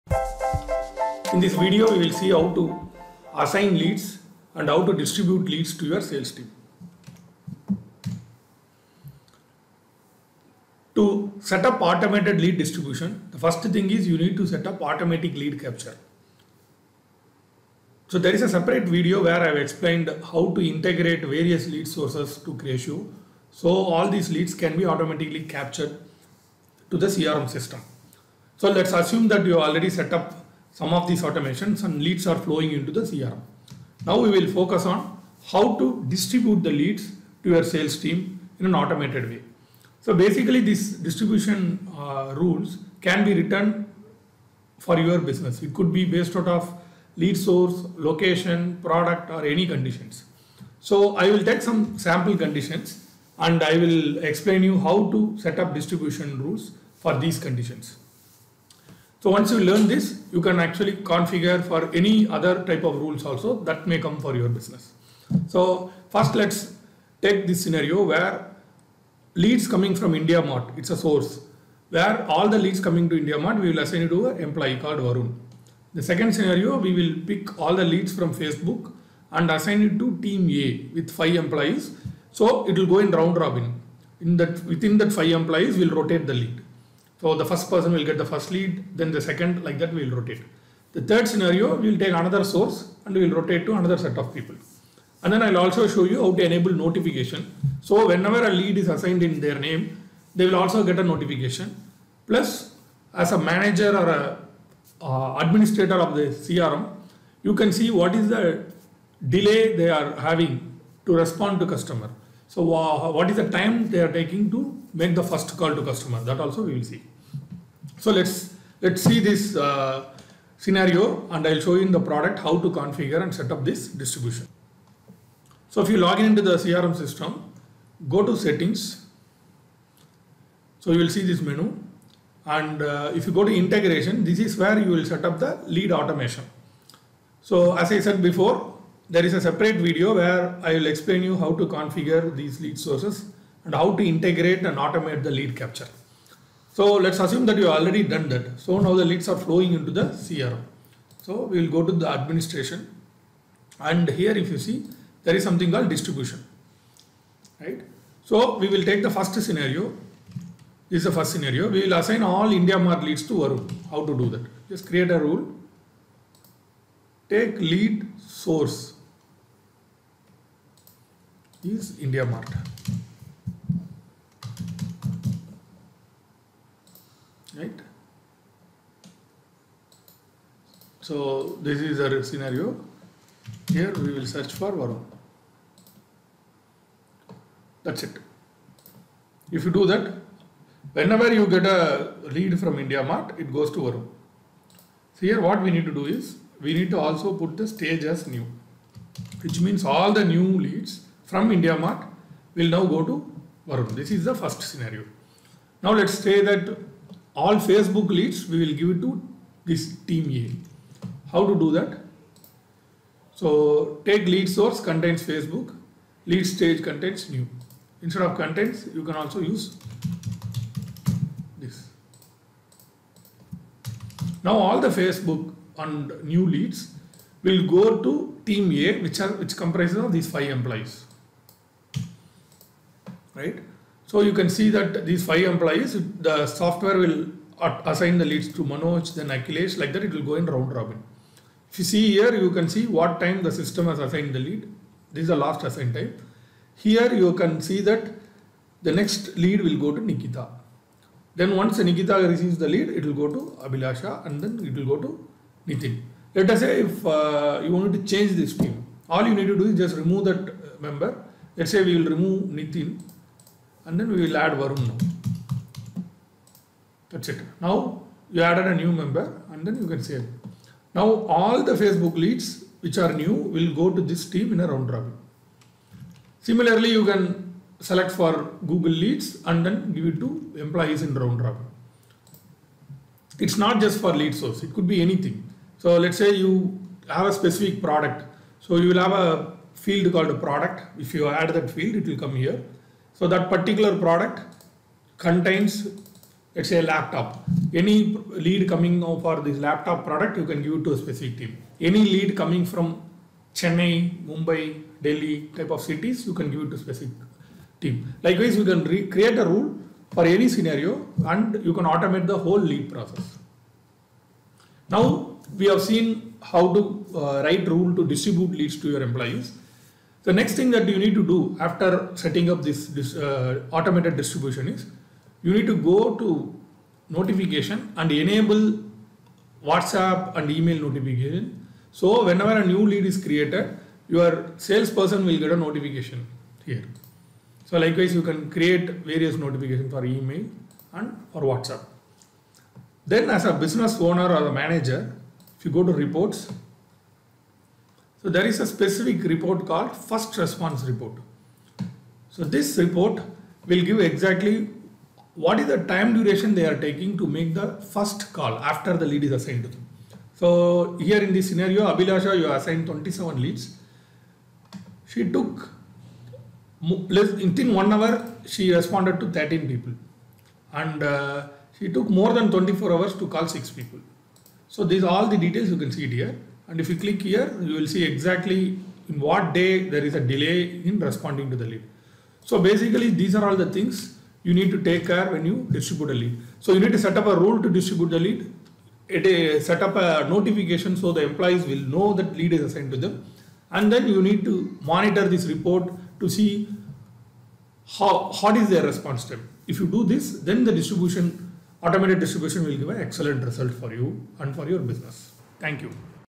Distributing Leads in Cratio CRM | Lead distribution in CRM
This video describes Lead distribution in Cratio CRM.

Want to get started with Cratio CRM? Get a free Demo today!!!